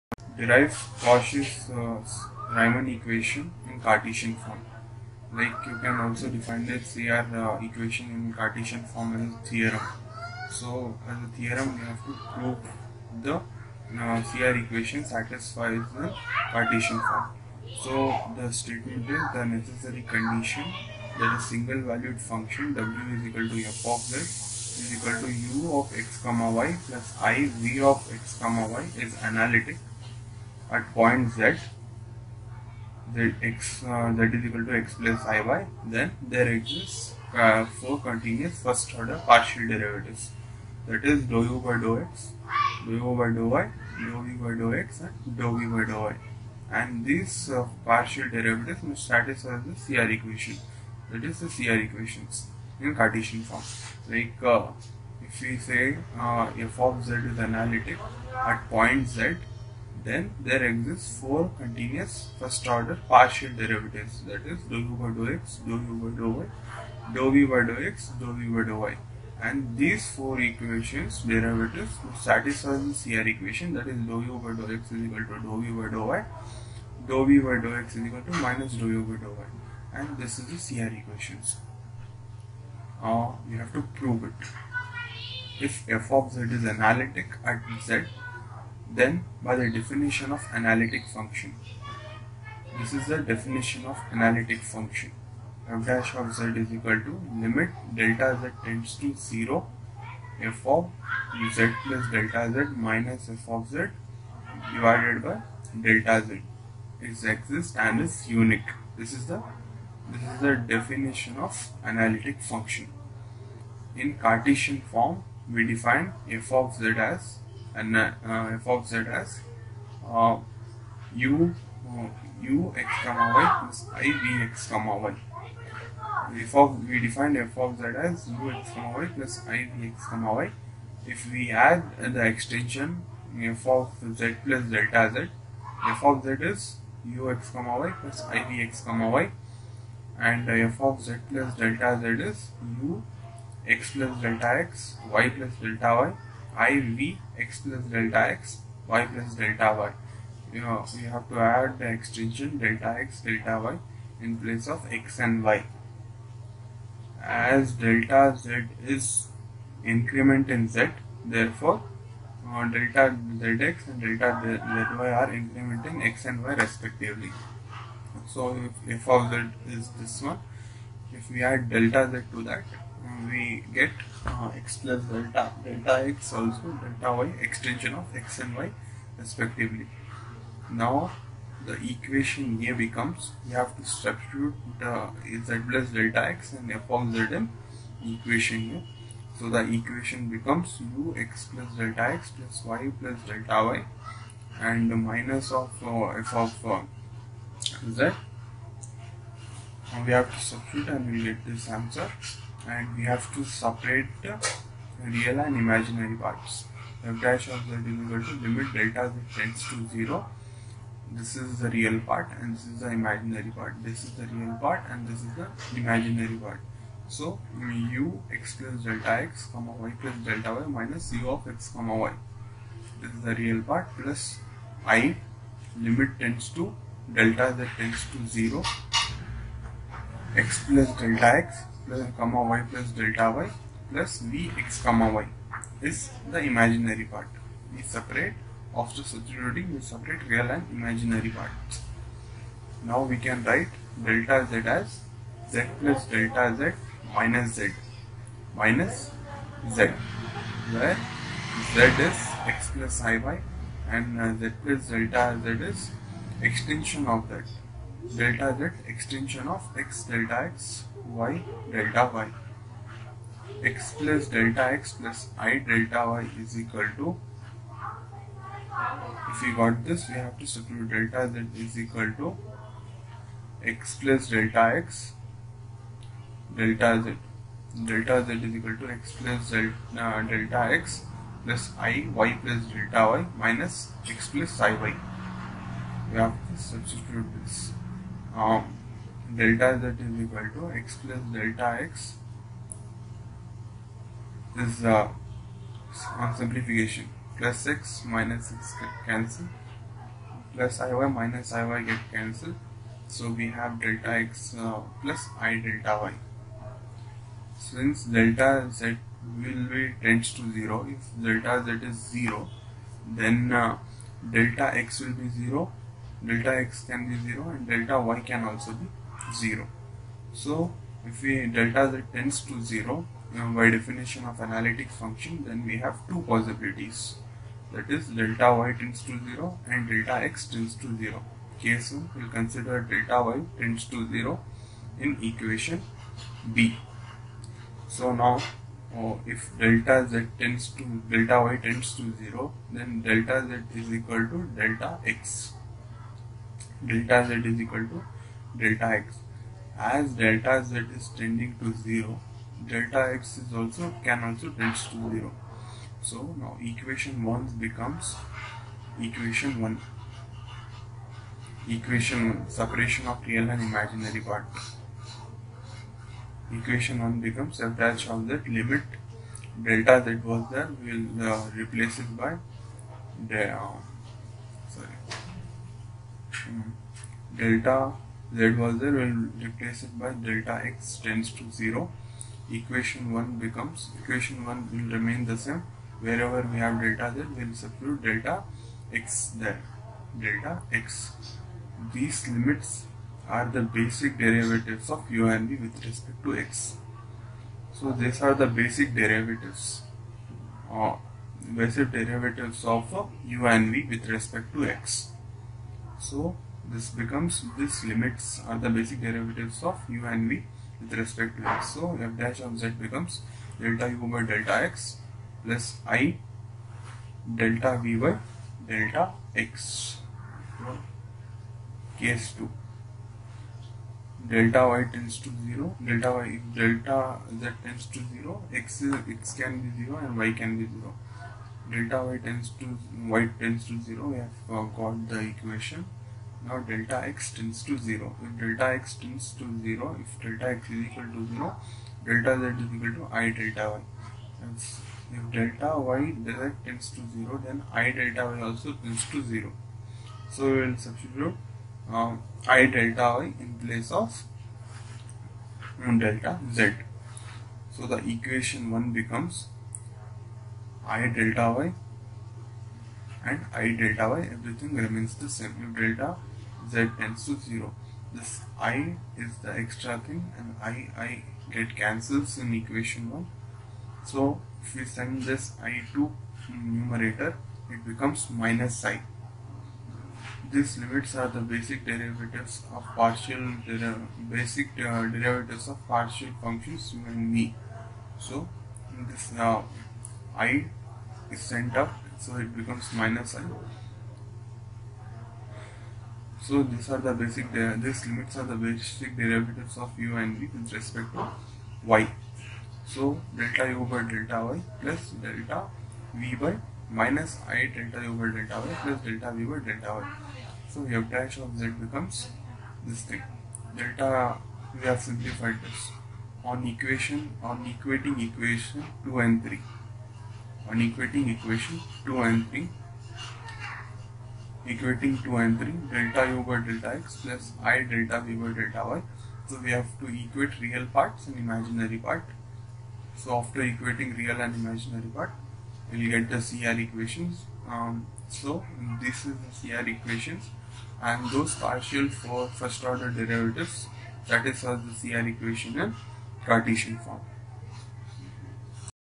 Derive Cauchy-Reimann equation in Cartesian form| VTU | 18MAT41 | M4 module -1| L-1
The video deals with derivation of Cauchy-Reimann equation in cartesian form